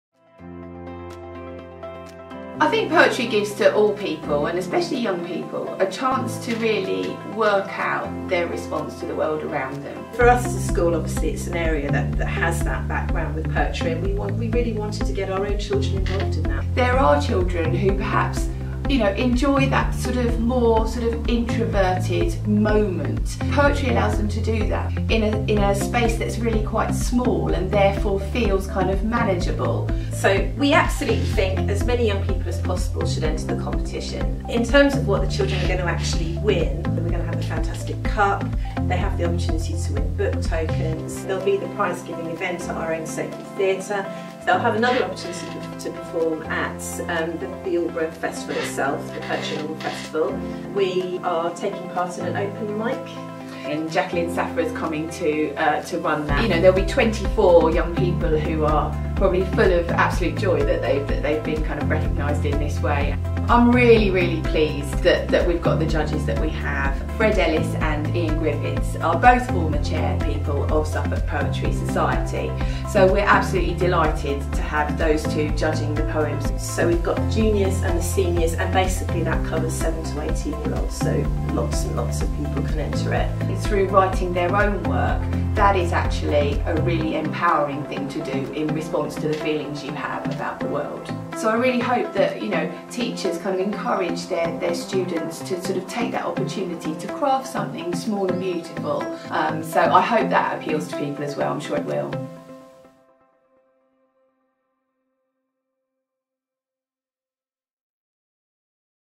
Woodbridge Young Poets Competition (2016) | Woodbridge School
Can you turn your passion into a poem?
If you are a creative young person in Suffolk, enter the Woodbridge Young Poets Competition.
for more details and fire your imagination!